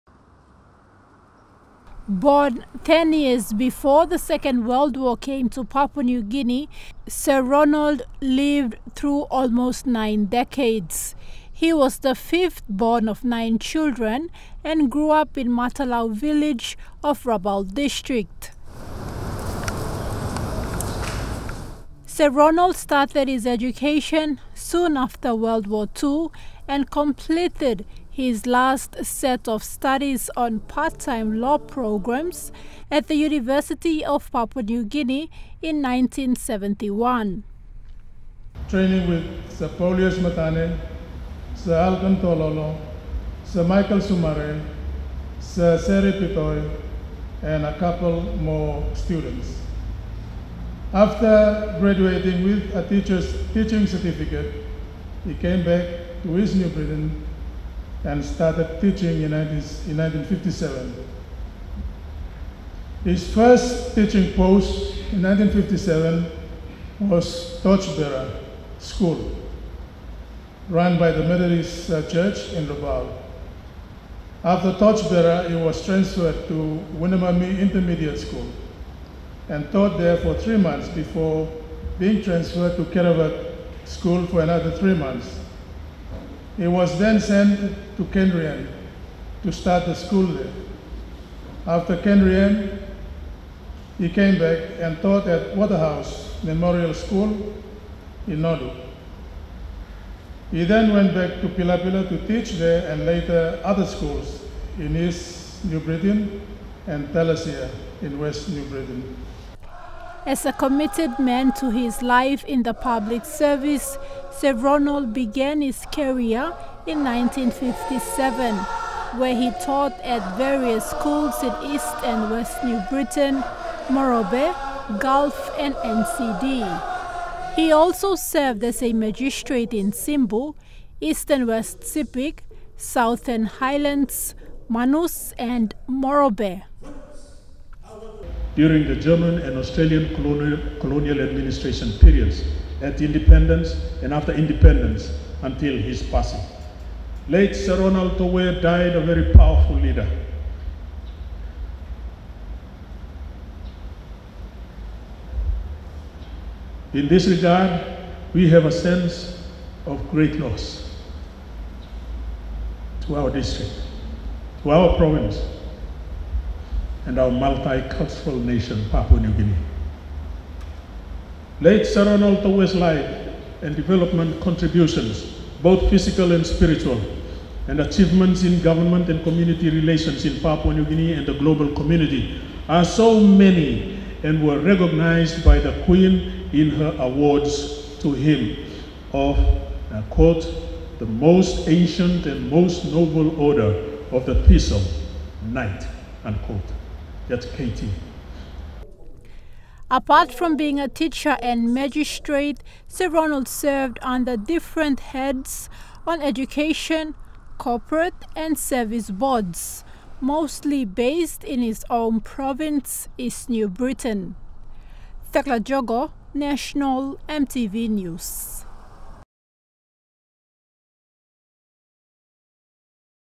Farewell Sir Ronald Tovue
The life of Late Ronald ToVue from Nonga village in East New Britain province was celebrated in a funeral service attended by immediate family, relatives, close friends and former colleagues.

Born on 17th February 1933, Sir Ronald was a senior statesman who held various jobs as teacher, magistrate and economist.

He passed away on 25th December 2021 and is survived by three children and eight grandchildren.